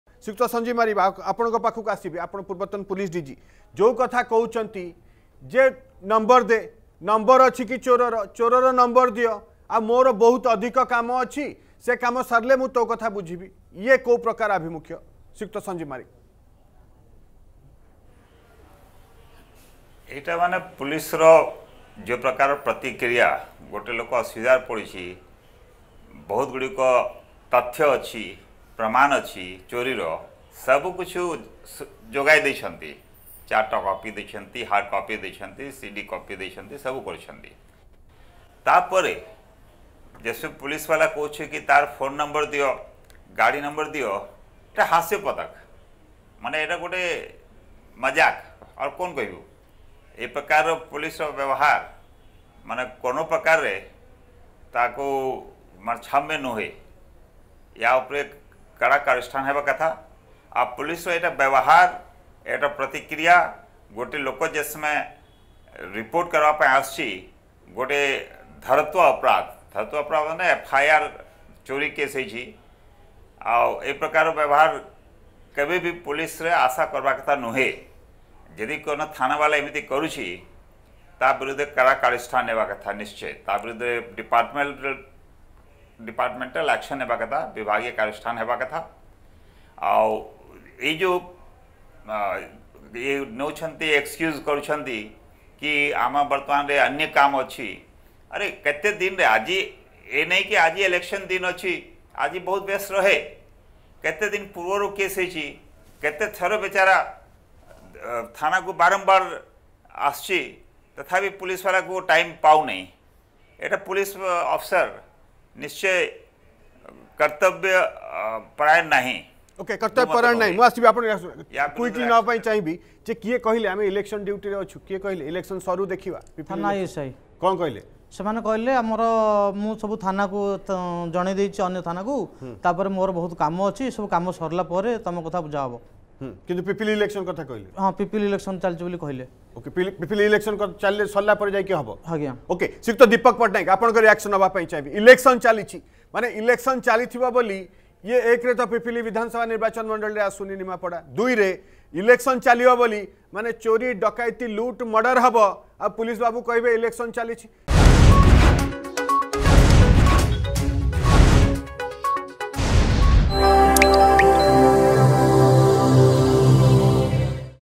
Complaint against Nimapada Police | ArgusNews
Such behavior by the police is ridiculous. Action should be taken in such cases: Sanjeev Marik, former Police DG